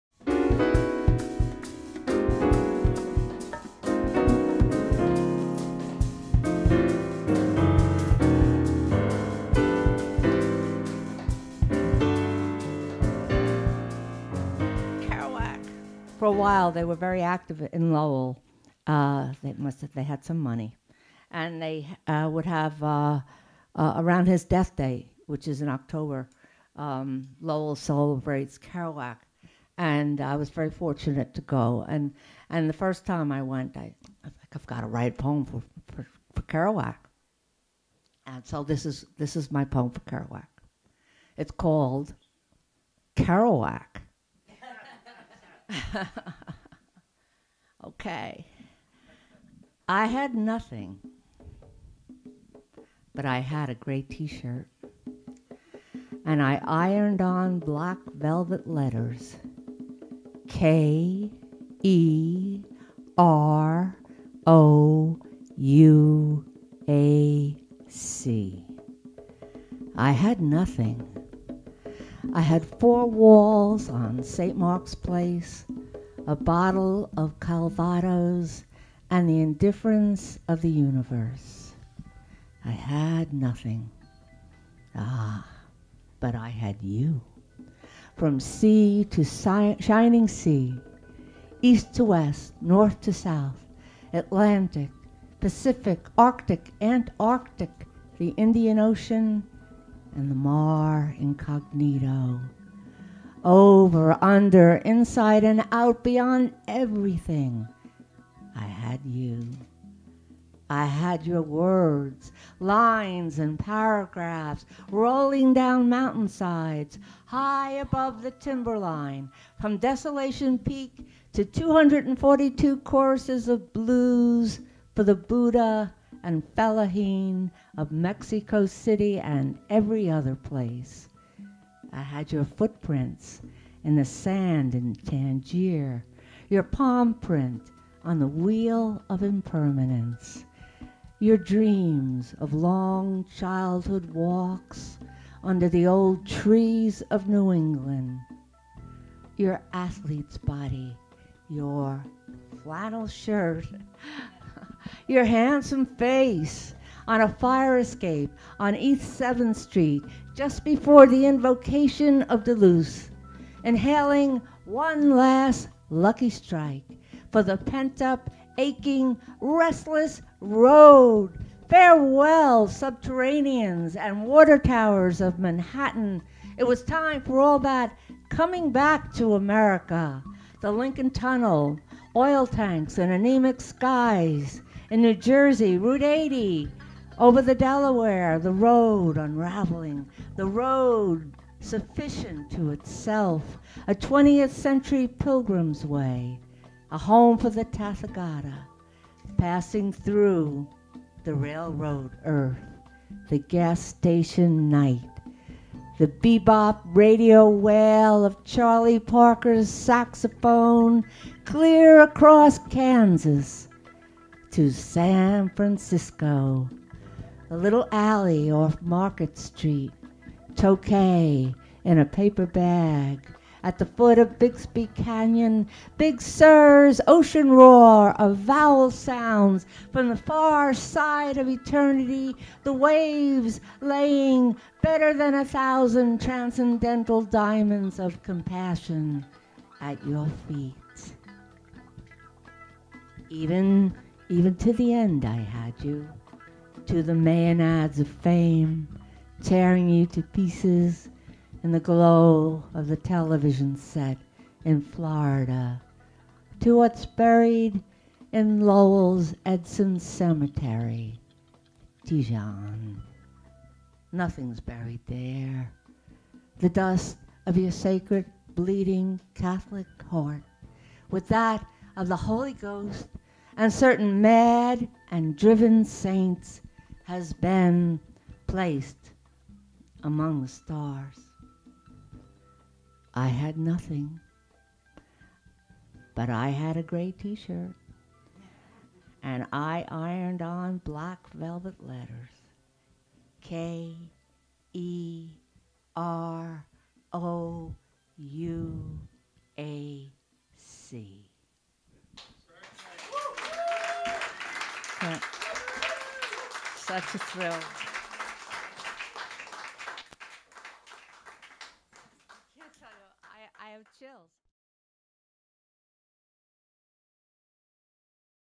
Mister WA presents : 'Kerouac' with Poet Janet Hamill
Recorded LIVE Wednesday June 21st, 2017 at The Beat Museum, North Beach, San Francisco. Music by Andy Woodhouse